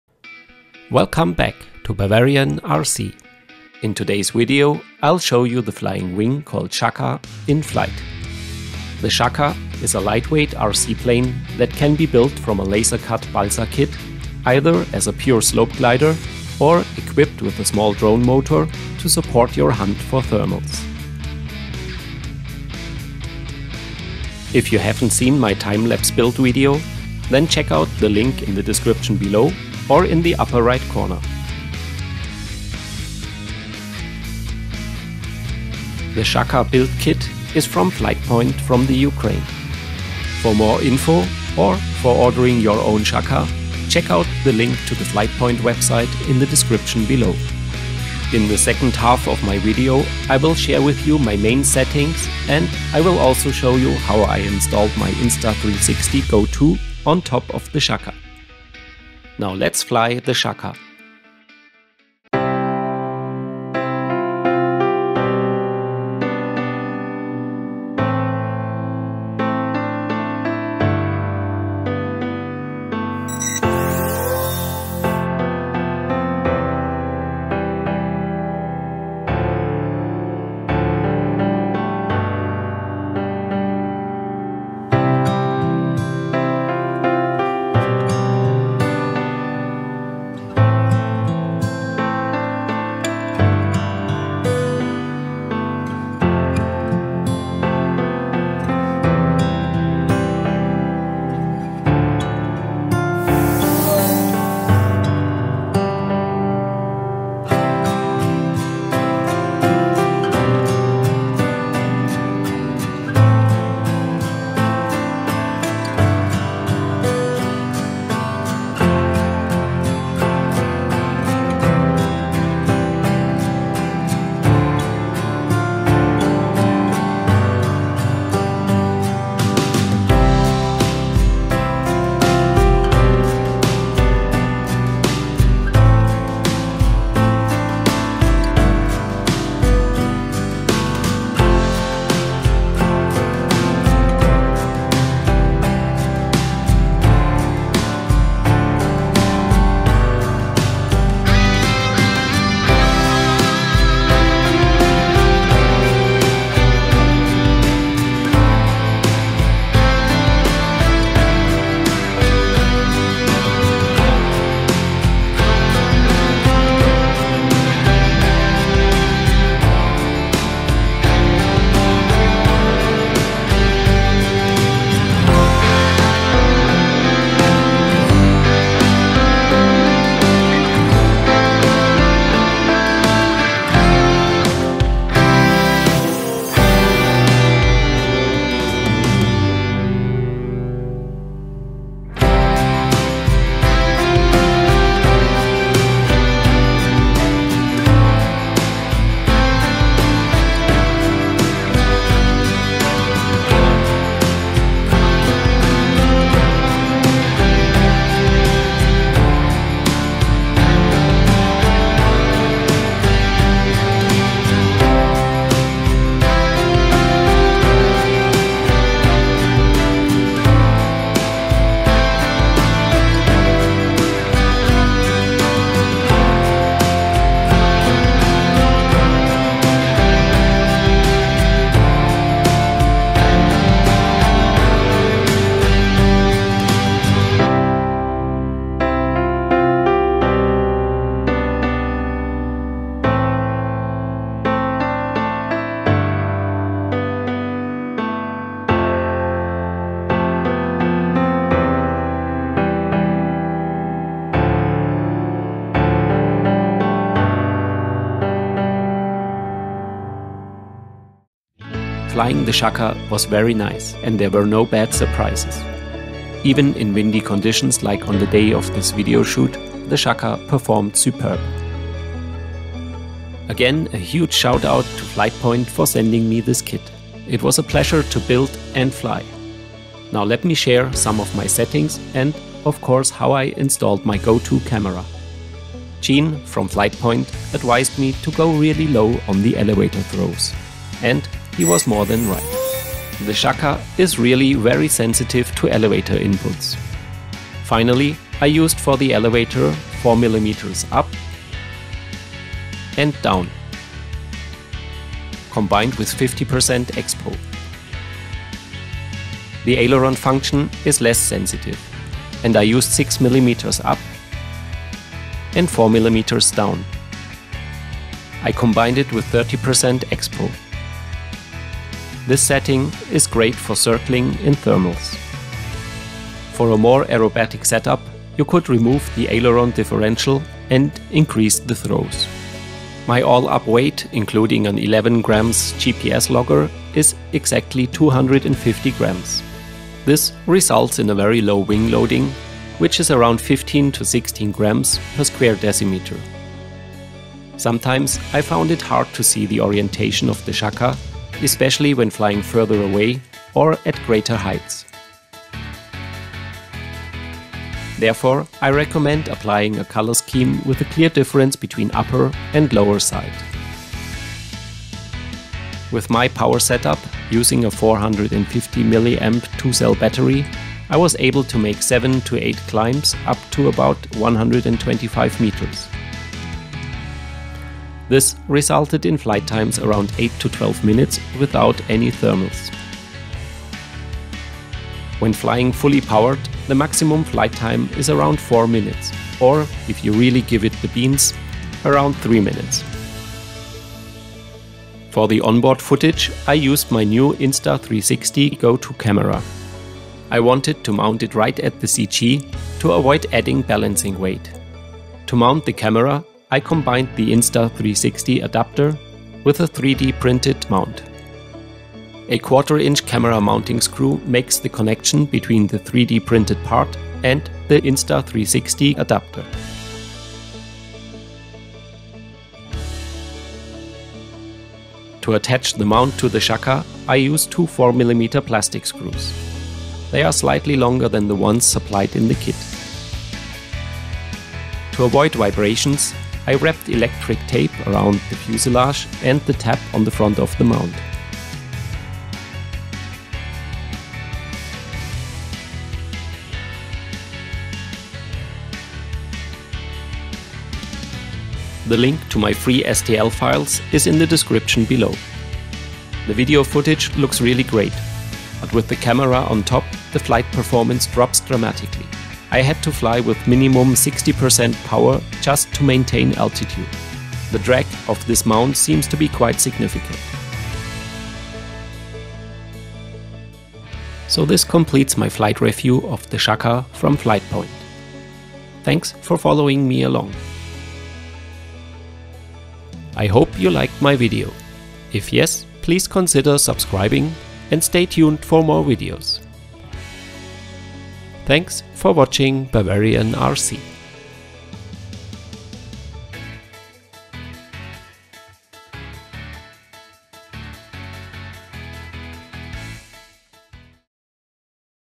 Flying the Shaka RC plane
In this video I will fly the Shaka from FlightPoint and share my tips & tricks around this flying wing. The Shaka features a classic balsa construction, reinforced with carbon fiber tube spars and leading edge. The kit consists of high quality laser cut balsa and plywood parts, carbon tubes and a complete accessory set.

Link to my free STL file for the Inst360 Go 2 mount:
1/4" camera mounting screw
Insta360 Go 2 Adapter Set

Link to my free STL files for an alternative GoPro mount:
Attach mount with four 3x12 universal screws (pan-head) to basis.

Specs:
Wingspan: 965 mm
Length: 440 mm
Weight (AUW): 250 g (in my configuration)
RC functions: elevator, ailerons, motor

My configuration:
Motor: T-Motor F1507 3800 kV
Prop: Graupner Carbon Folding Prop 6x3"
Battery: Tattu 2S 450mAh 75C, Long, XT30
ESC: Graupner Brushless Control + T 18
Receiver: Graupner GR-12 HoTT - 2.4 GHz, 6 channel
Servos: 2x Emax ES08D II
Spinner: VM 25mm Spinner for M5 shafts (Vladimir Models)

Covering film:
ORALIGHT transparent orange
ORALIGHT transparent blue
ORACOVER white
ORACOVER Hotmelt Adhesive (960)
ORACOVER Thinner for Hotmelt Adhesive (980)

Vinyl:
d-c-fix glossy black vinyl

Paint for fuselage:
Clou Schnellschleifgrund (wood primer)
Dupli Color Platinum white semi gloss

Cutting machine:
BROTHER ScanNCut SDX1000

Cameras:
Mavic Air
Sony HDR-CX730

Microphone:
Rode NTUSB